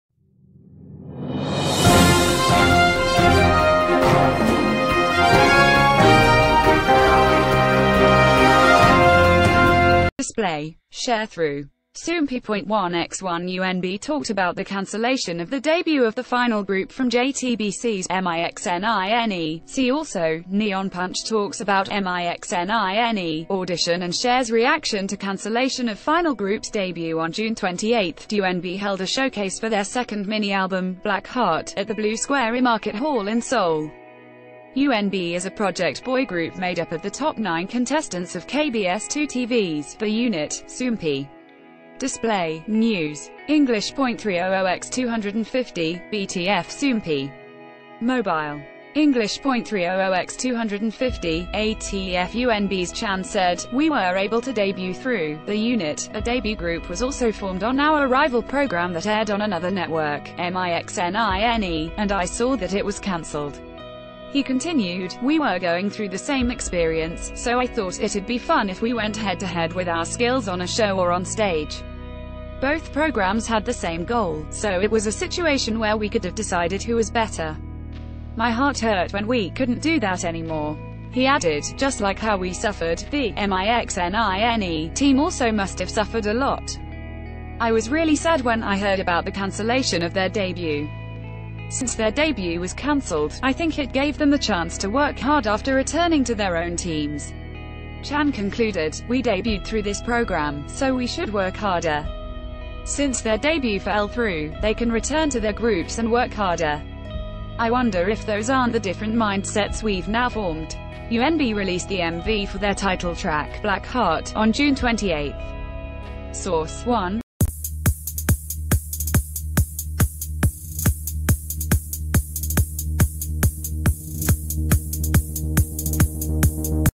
UNB Shares Thoughts On The Cancellation Of "MIXNINE" Final Group Debut
Display. Sharethrough. Soompi.1x1 UNB talked about the cancellation of the debut of the final group ...